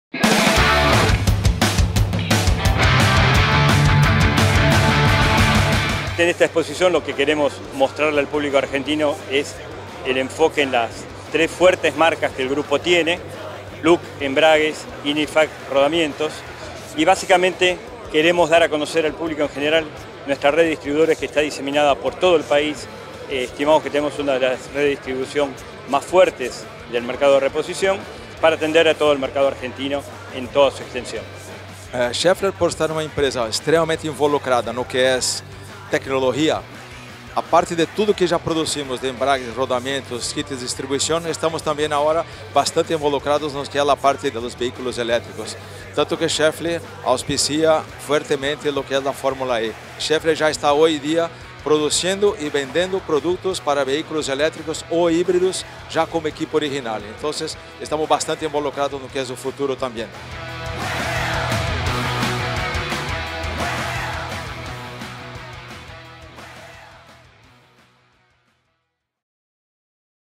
Institucional Schaeffler: Automechanika Bs As 2018
Schaeffler se enfoco en sus tres marcas importantes del Grupo y su red de Distribuidores en el país.